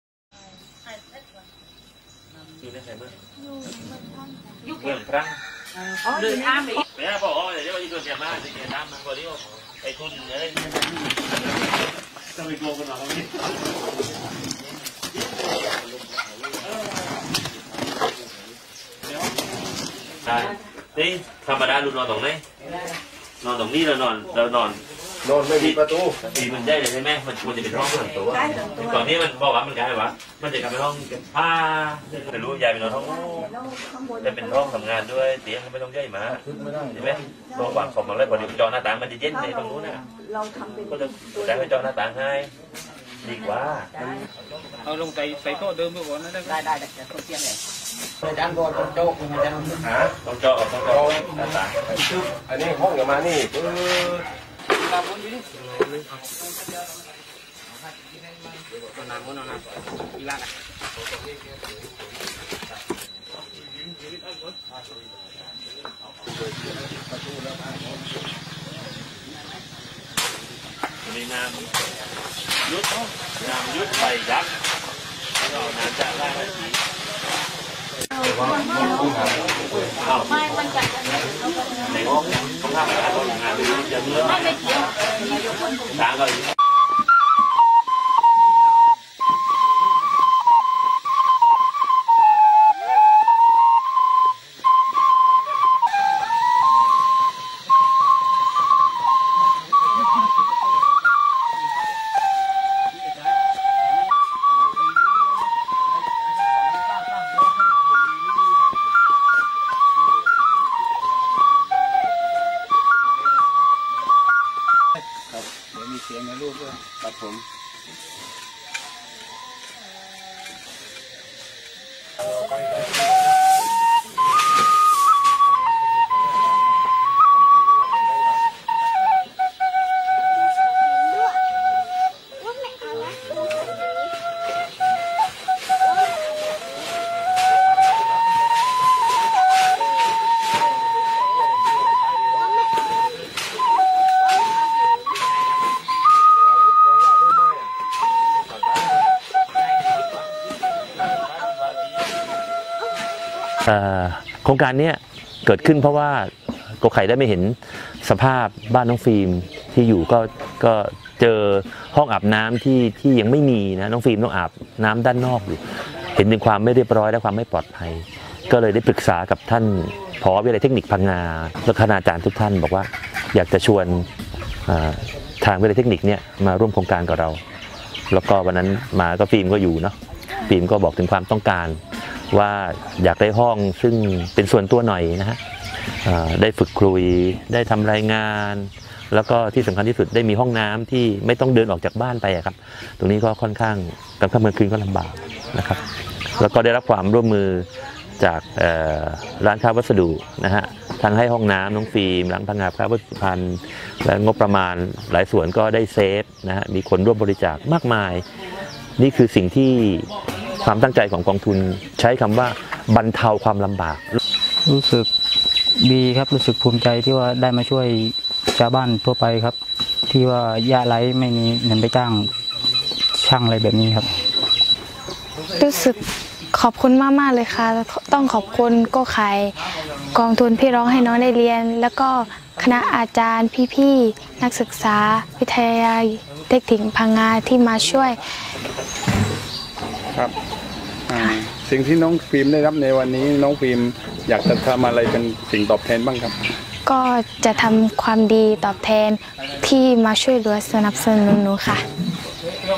สุดดีงาม!นักร้อง-นักศึกษารวมพลังช่วยซ่อมบ้านให้เด็กหญิงลูกกตัญญู
2019/09/13 สุดดีงาม!นักร้อง-นักศึกษารวมพลังช่วยซ่อมบ้านให้เด็กหญิงลูกกตัญญู เคยโชว์เป่าขลุ่ยแลกผ้าอ้อมให้พ่อพิการ

พังงา - นักร้องชื่อดังชาวพังงา “โกไข่” ผู้ก่อตั้งกองทุนพี่ร้องให้น้องได้เรียนพังงาร่วมกับนักศึกษาวิทยาลัยเทคนิคพังงา รวมพลังช่วยซ่อมบ้านให้เด็กหญิงลูกกตัญญู เคยโชว์เป่าขลุ่ยแลกผ้าอ้อมให้พ่อพิการ “น้องฟิล์ม”